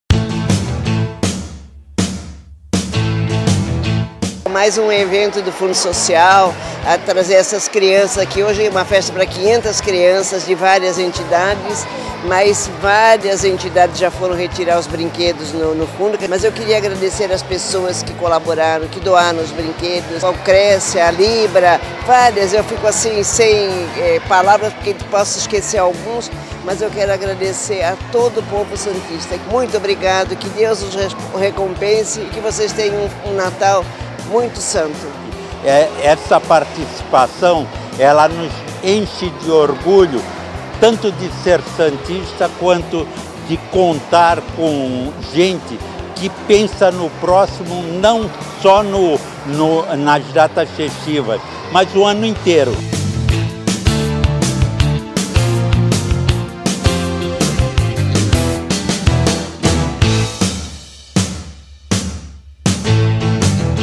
Festa de Natal integra entidades cadastradas no FSS
Cerca de 500 crianças participaram, nesta terça (15), da Festa de Natal do Fundo Social de Solidariedade (FSS), no Clube Internacional. A confraternização contou com a participação de 11 entidades cadastradas no órgão da Prefeitura. Todas as crianças receberam lanches e brinquedos.